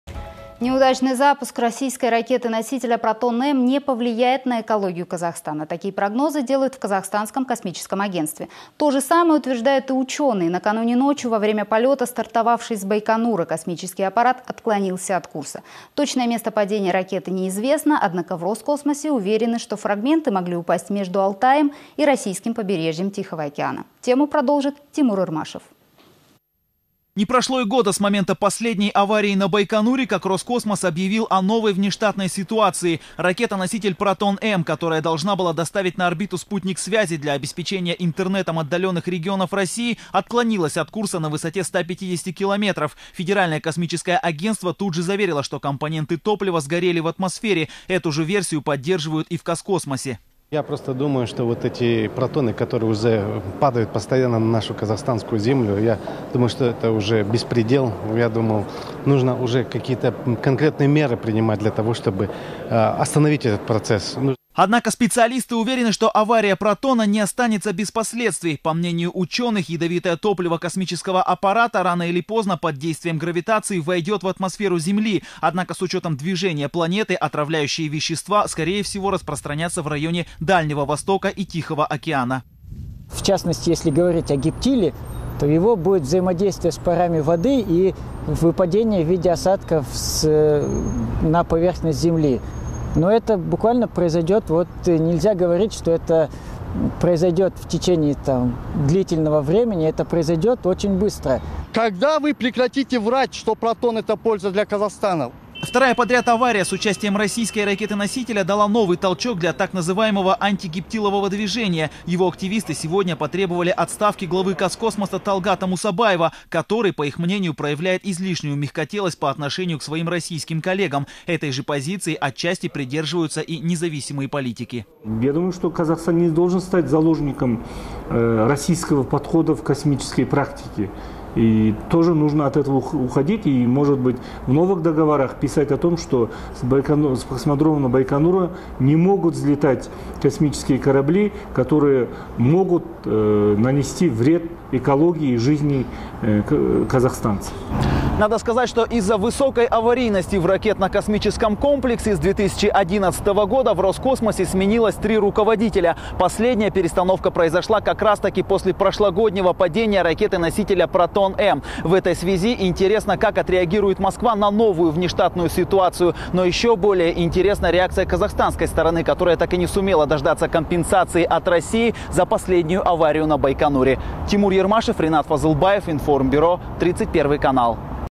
Неудачный запуск "Протона" не повлияет на экологию
Неудачный запуск российской ракеты-носителя «Протон-М» не повлияет на экологию Казахстана. Такие прогнозы делают в Казахстанском космическом Агентстве. То же самое утверждают и ученые. Накануне ночью во время полета стартовавший с Байконура космический аппарат отклонился от курса. Точное место падения ракеты не известно, однако в "Роскосмосе" уверены, что фрагменты могли упасть между Алтаем и российским побережьем Тихого океана.